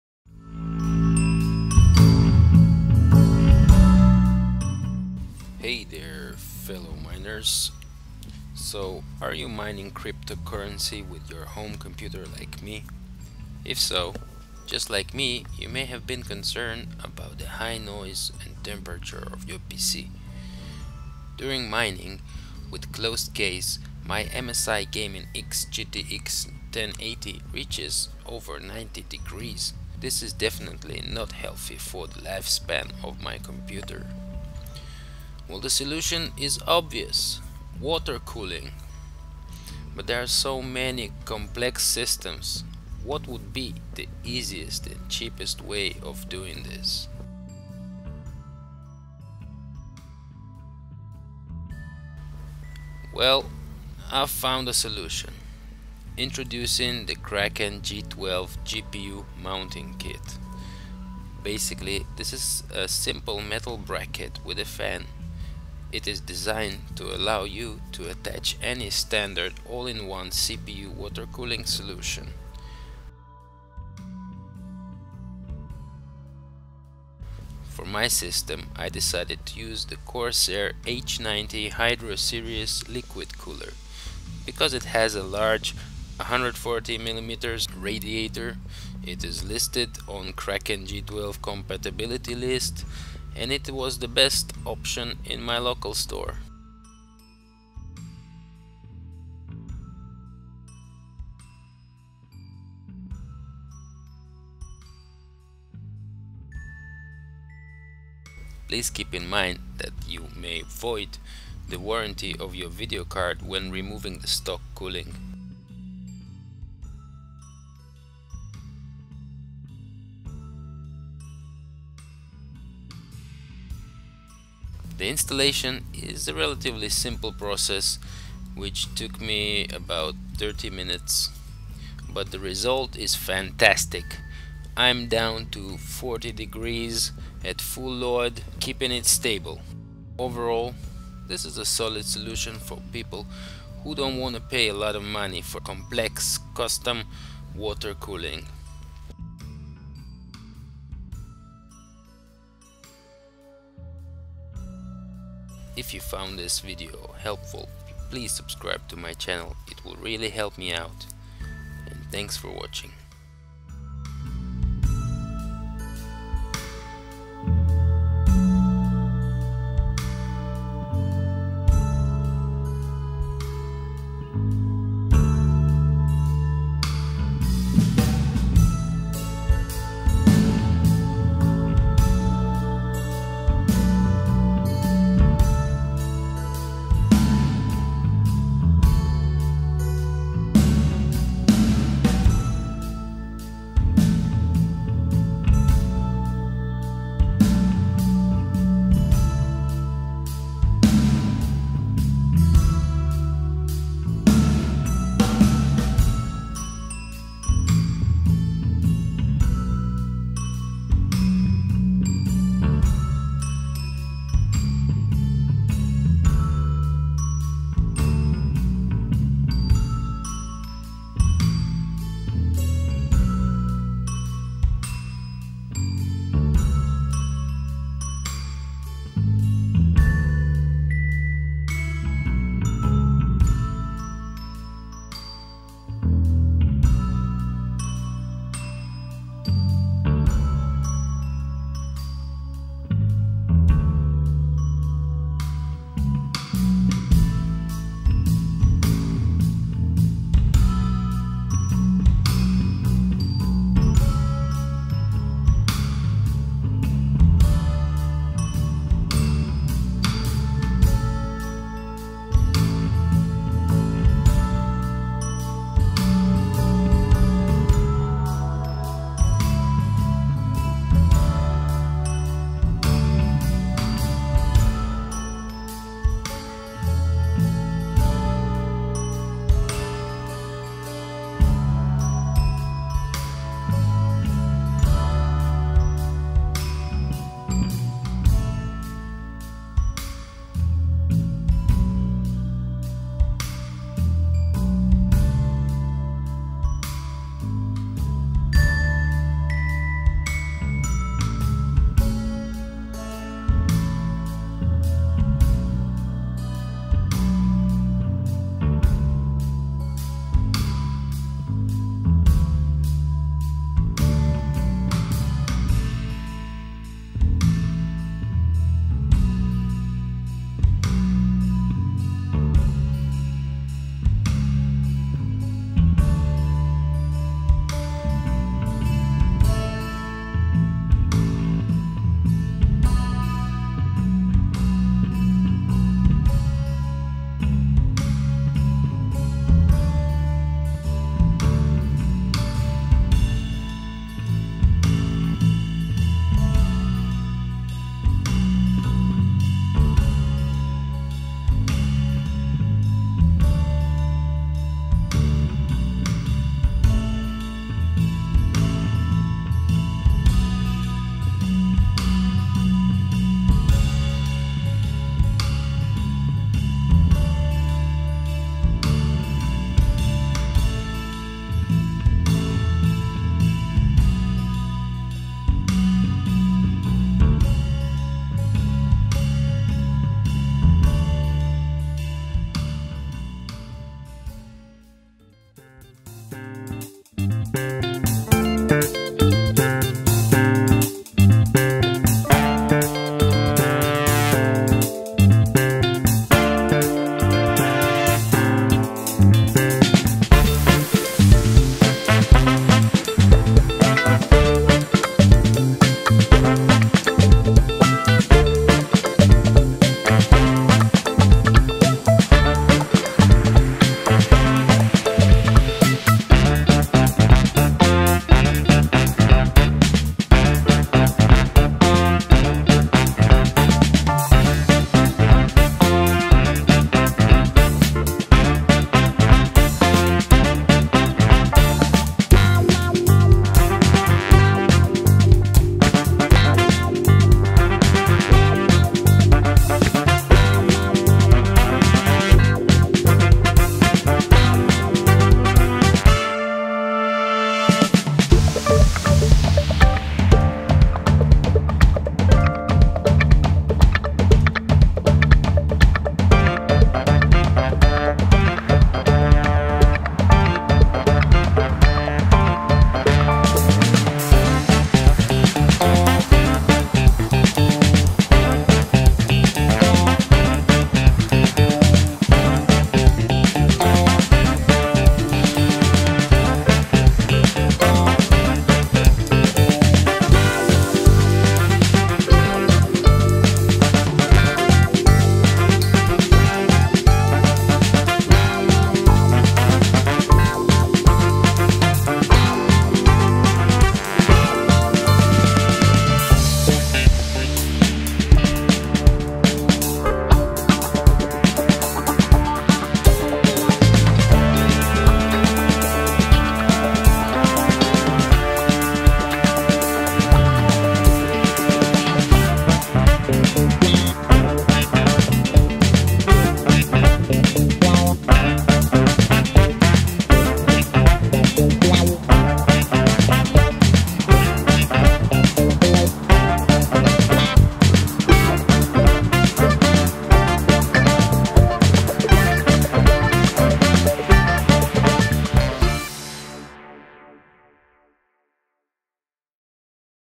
Kraken G12 with GTX 1080 installation guide (4K)
Replacing stock msi gaming x gtx 1080 cooling with corsair h90 and kraken g12.
This computer mines crypto when it's not in use.
The temperatures never rise above:
CPU: 60 (stock AMD cooling)
GPU: 41

CPU: Ryzen 7 1700
Case: Antec Razer Cube